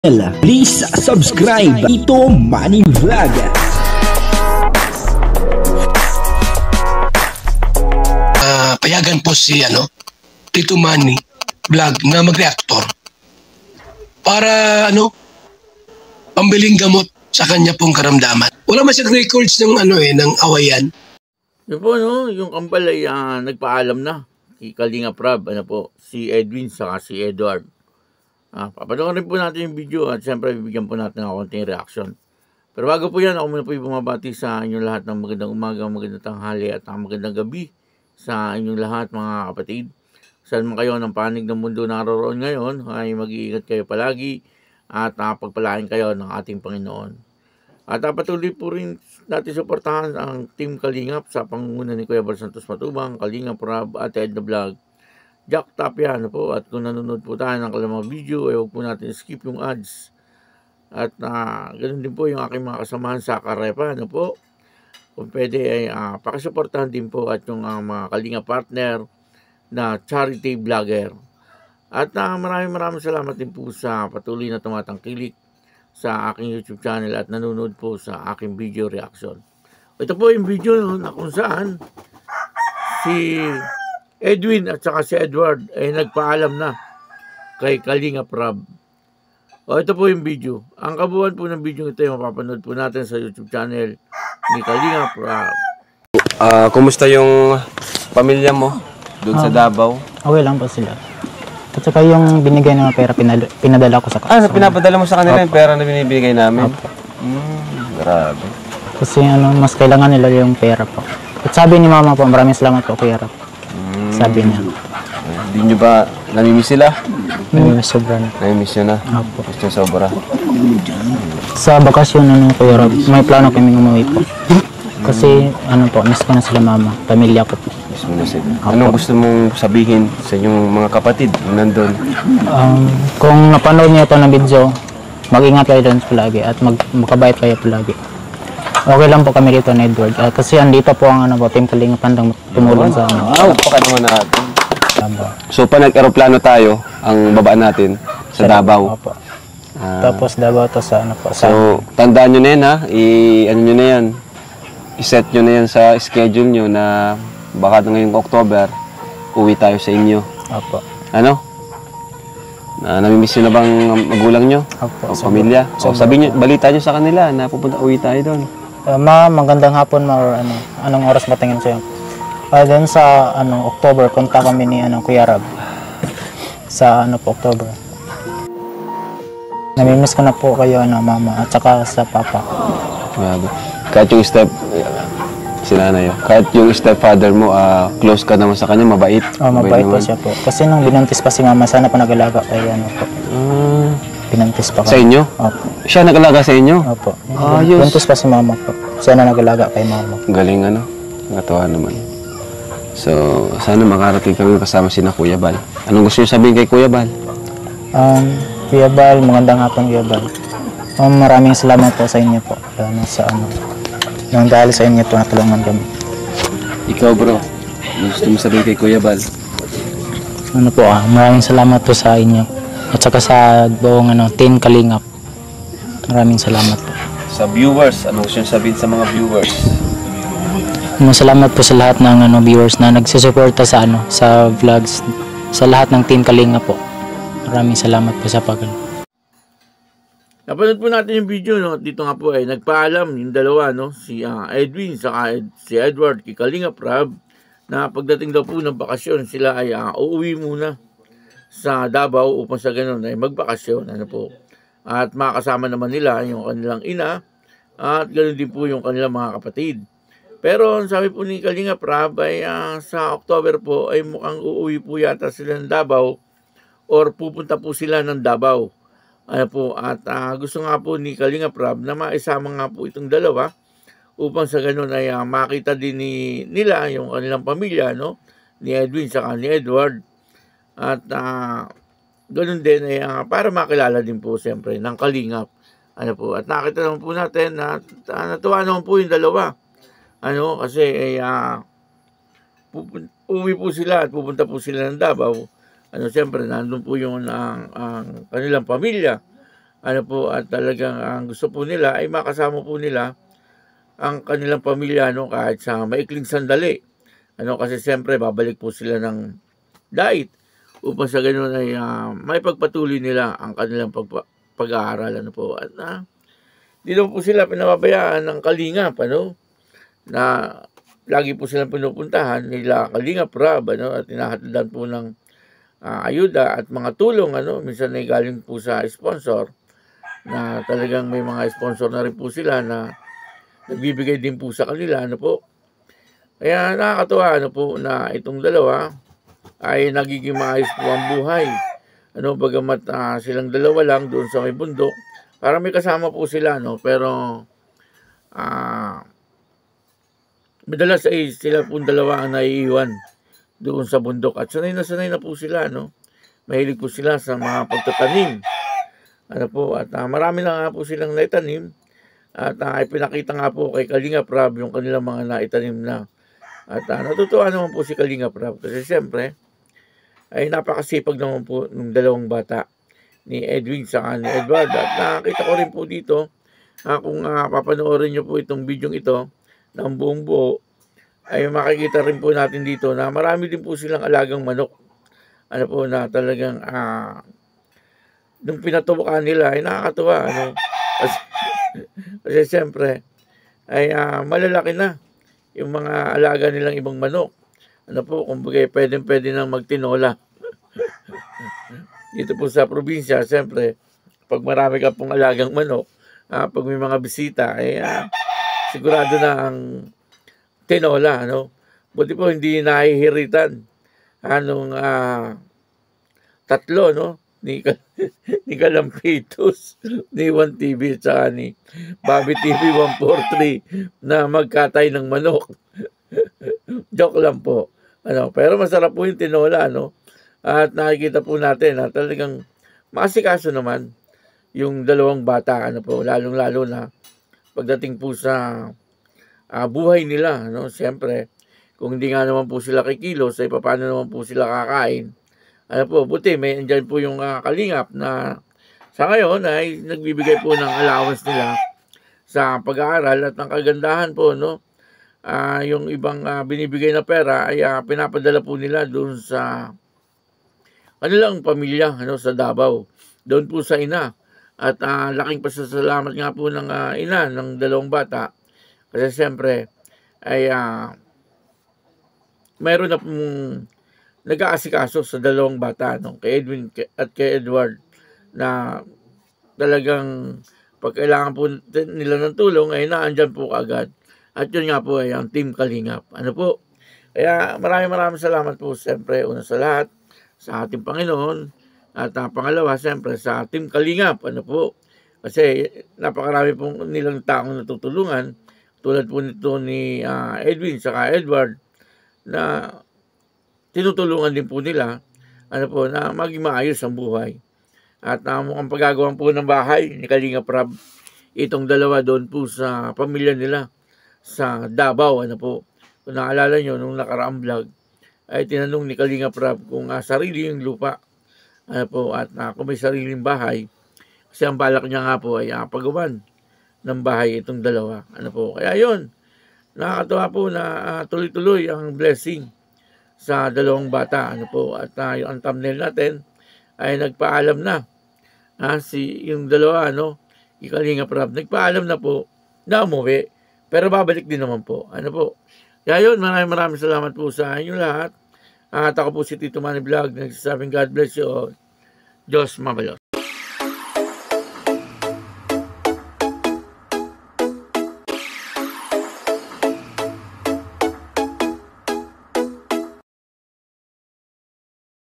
kambal na SI Edwin at Edward nagpaalam na#kalingaprab#valsantosmatubang
kambal na SI Edwin at Edward nagpaalam na Kay kalingap Rab